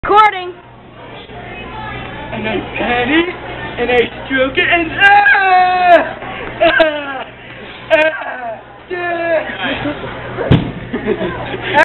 Wills gets Jumped
Ryan ( me ) punches will in the head cause he ripped my bagel in half.